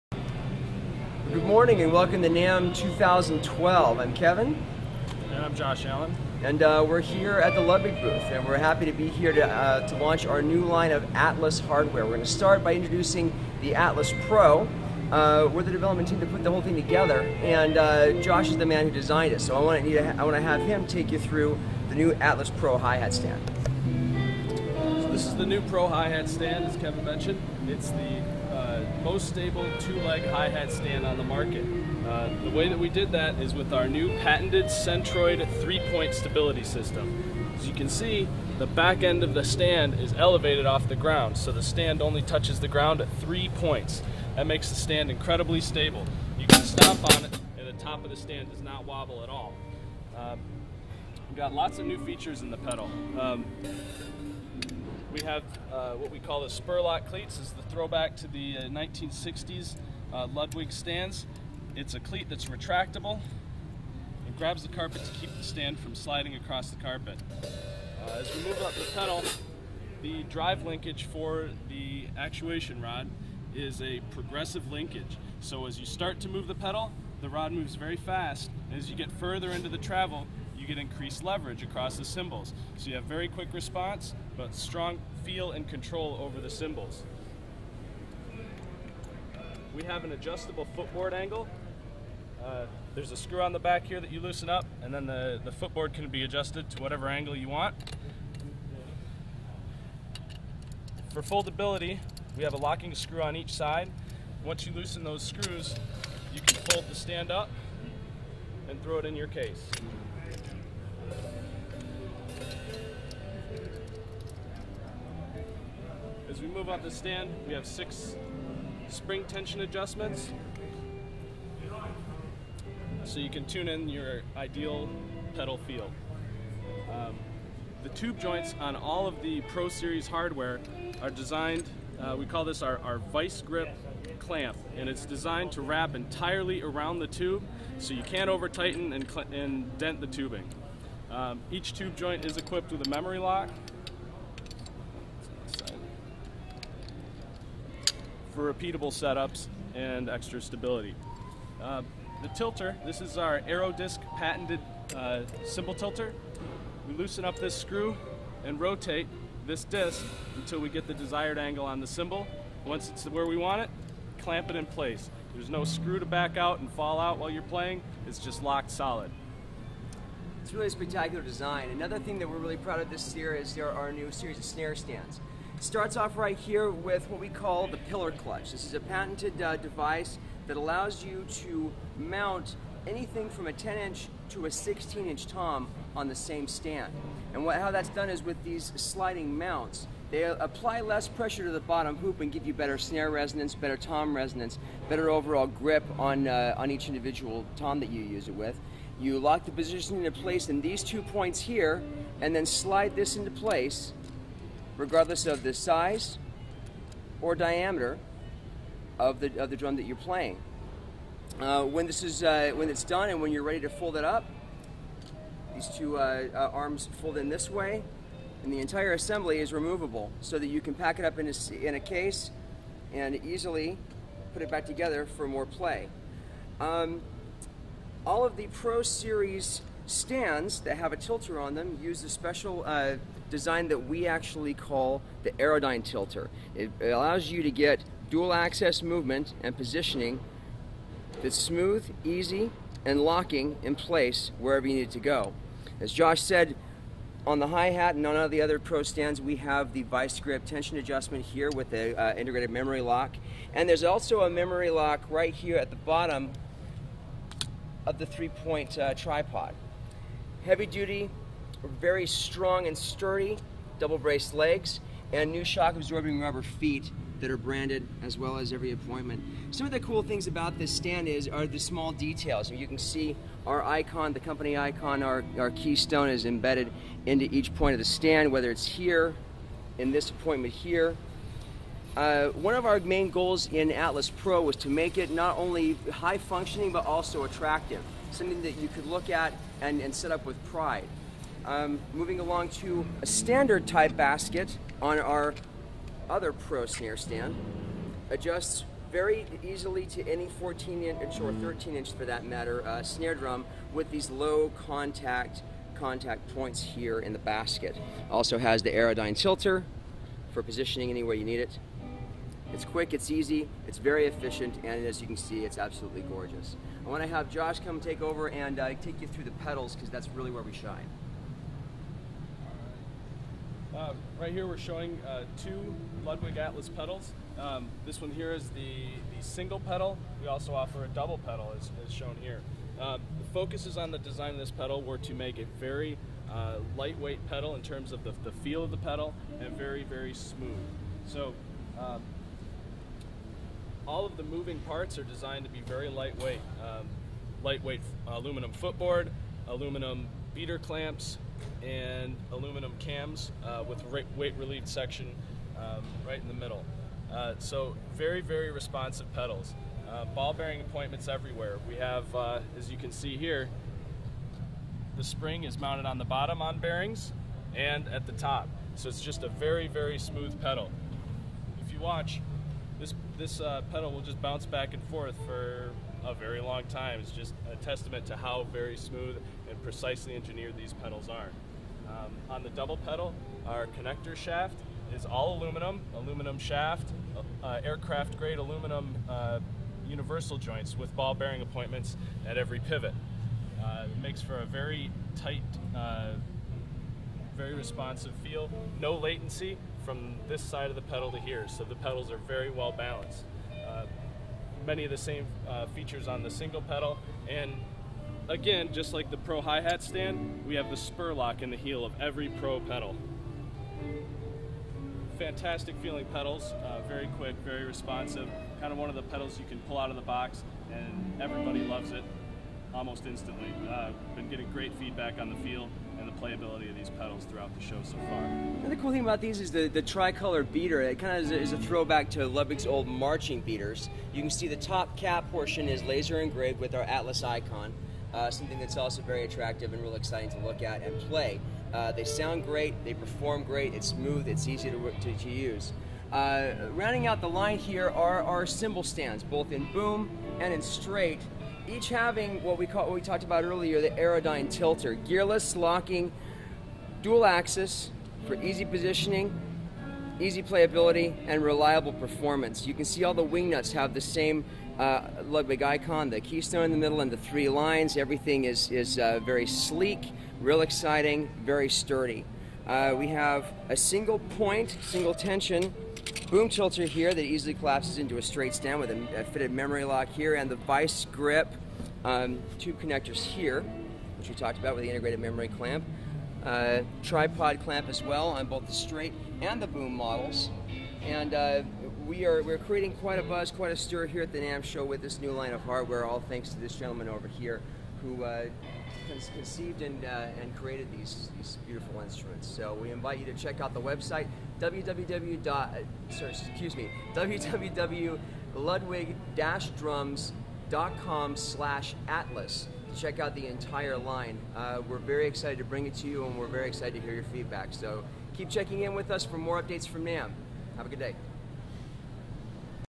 Ludwig_Atlas_Pro_Hardware.flv
Ludwig Atlas Pro Hardware is designed for the touring professional. Heavy-weight, durable and engineered for high performance while withstanding full-impact play and frequent travel. Available at 2112 Percussion.